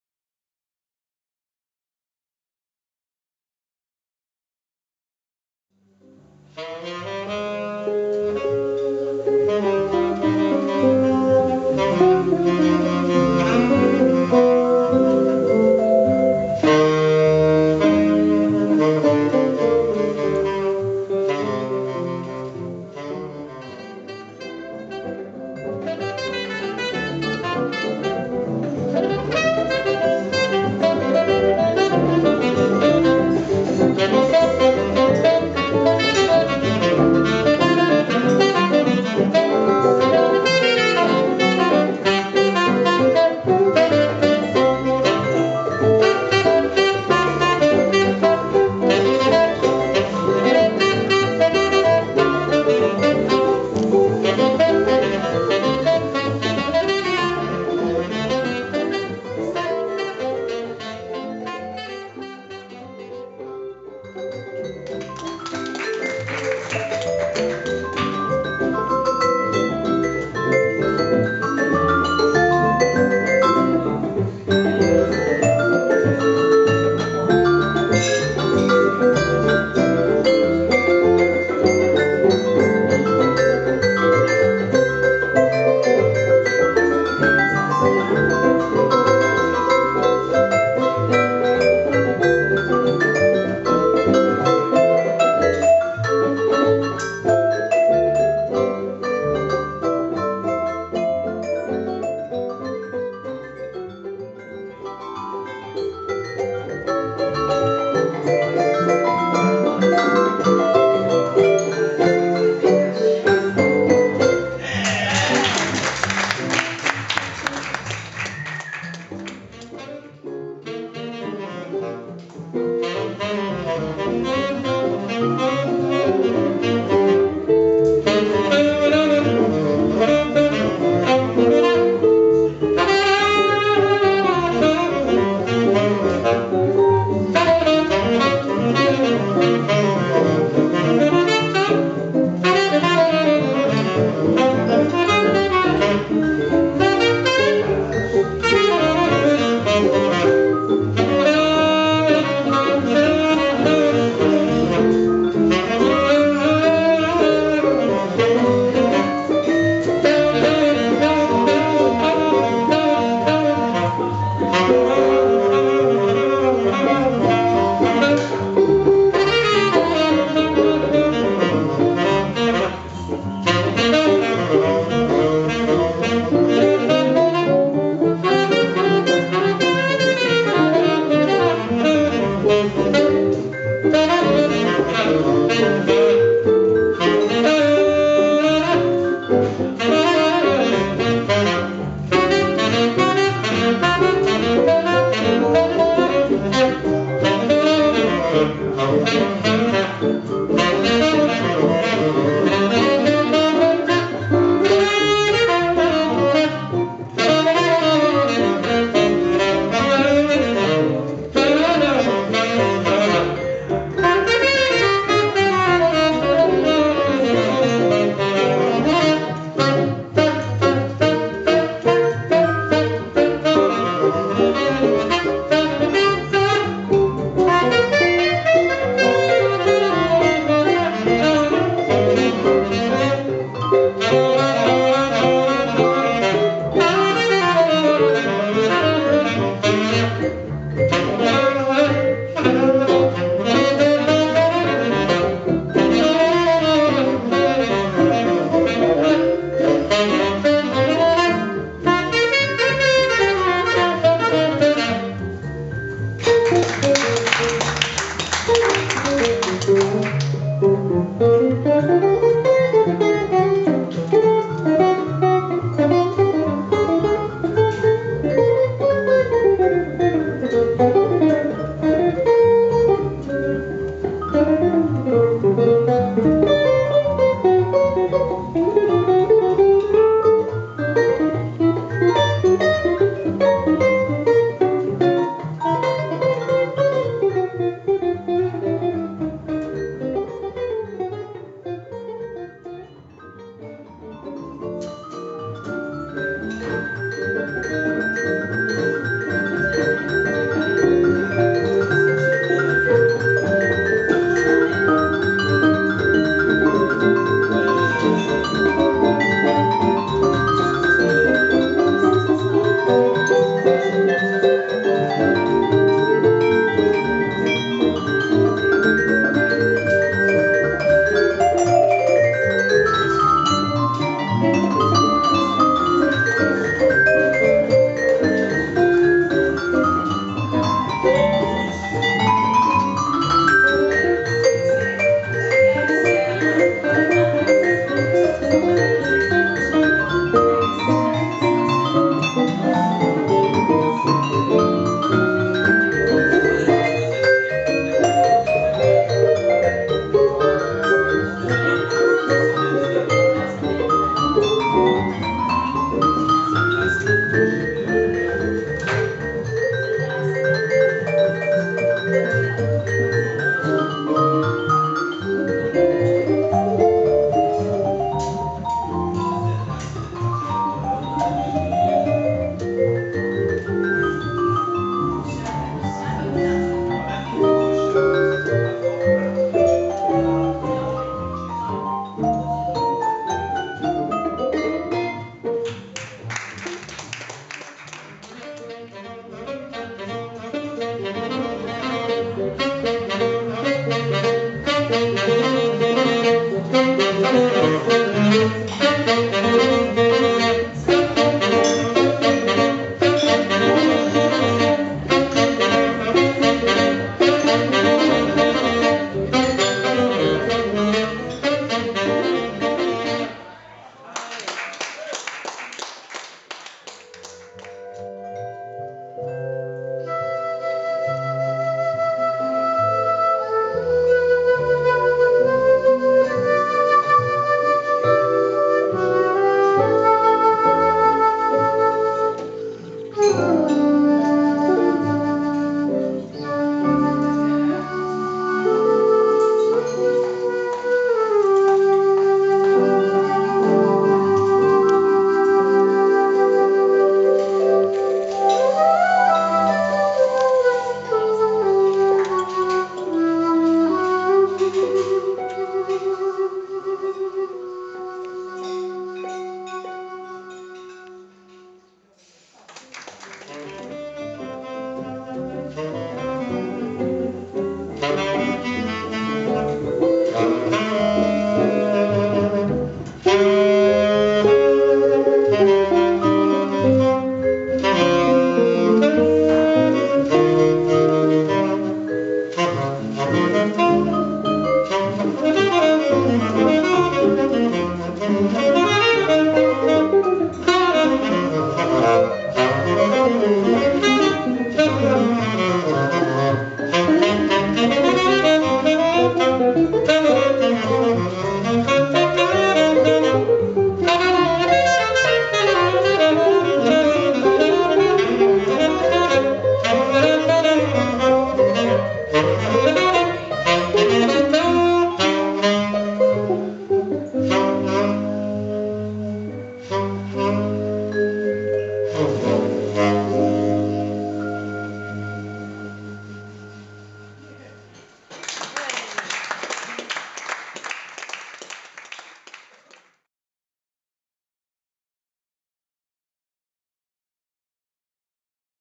Jimmy Hastings Frank Sinatra Saxophonist, Roan Kearsey-Lawson Vibes Player, Ramada Encore Hotel
Live from the Maritime Jazz Festival 2011 Ramada Encore Chatham with Jimmy Hastings on Flute and Sax, Roan "Hampton" Kearsey-Lawson on Vibes, John Myhill on Guitar and John Richards on Bass.